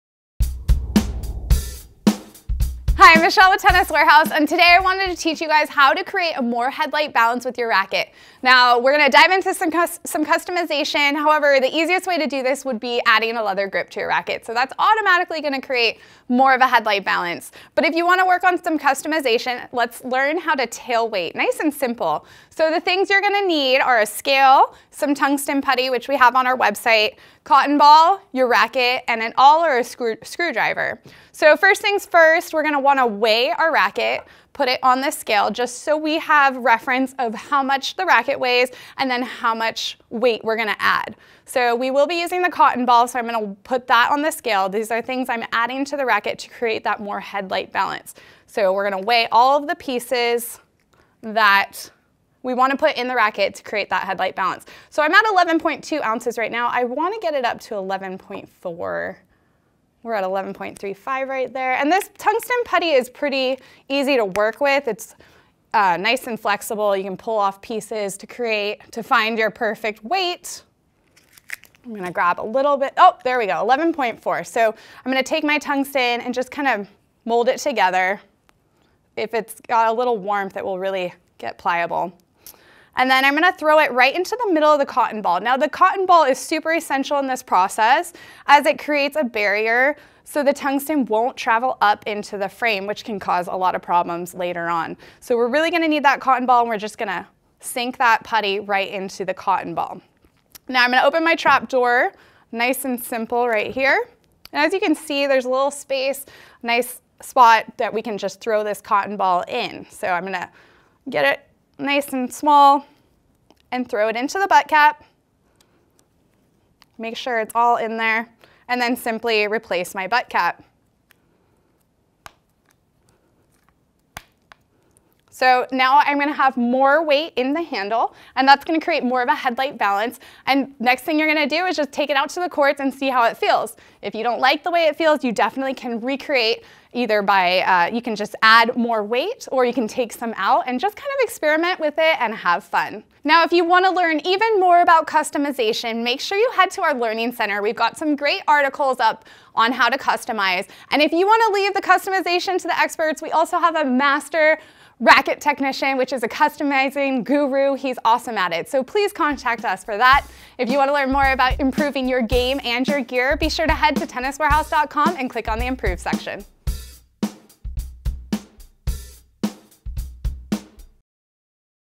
Improve Your Game: How to Customize Your Tennis Racquet (tailweight)
Take your tennis racquet to the next level with some simple customizing!
Looking to make your racquet more head light? Learn how to simply customize your tennis racquet and create a more head light balance by adding weight to the handle.